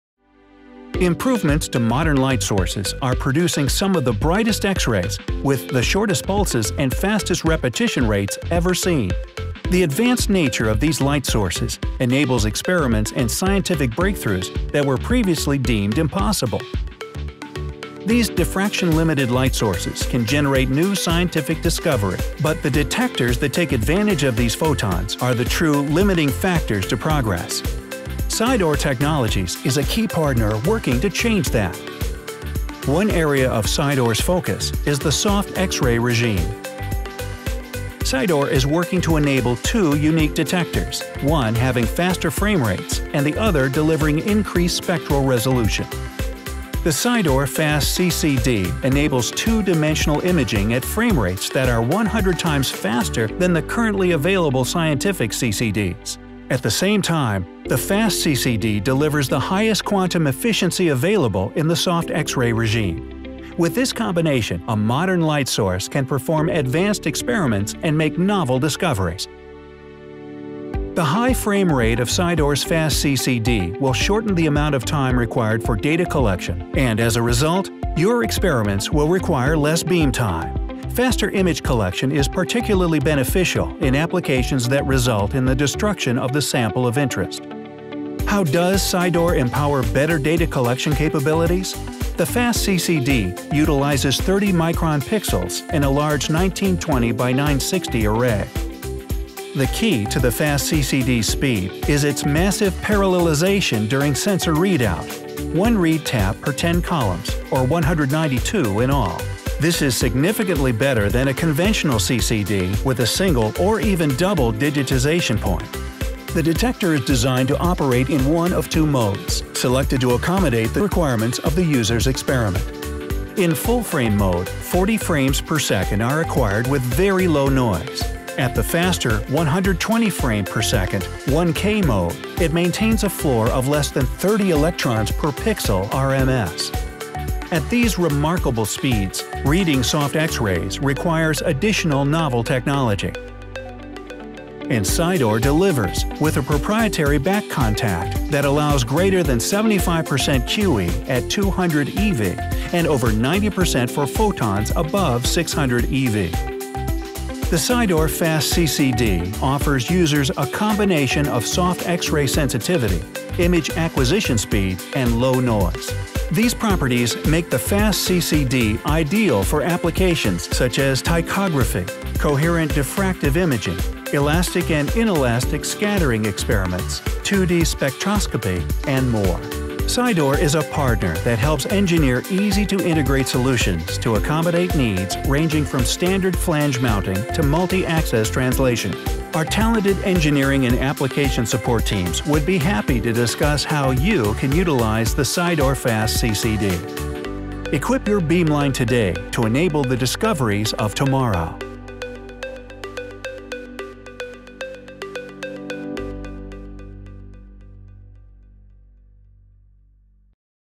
Sydor Technologies Fast CCD for Soft X-Ray Detection
The Fast CCD utilizes a unique window to allow capture of EUV to tender X-rays. When combined with on board electronics that acquire at 120fps – about 100 times faster than others – the combination of soft and fast is the best option for many beamline applications.